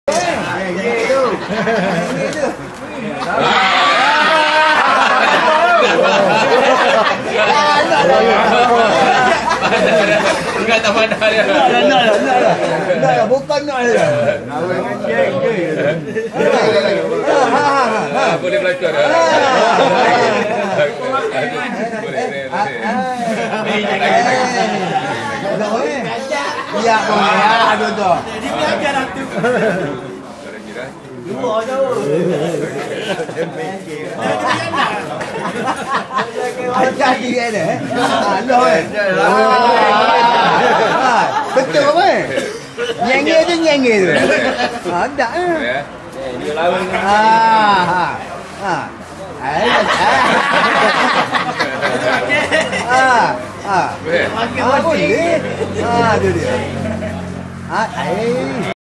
Video throwback Dato Seri Mustapa Mohamed bermain Dam Aji bersama Pak Tua di kampung Panglima Bayu
Video throwback Dato Seri Mustapa Mohamed bermain Dam Aji bersama Pak Tua
di kampung Panglima Bayu, Tanah Merah, Kelantan. Aktiviti sebegini sudah biasa bagi seorang pemimpin berjiwa rakyat seperti beliau. Satu usaha yang wajib dicontohi oleh semua pemimpin.
sumber video FB RASMI :Mustapa Mohamed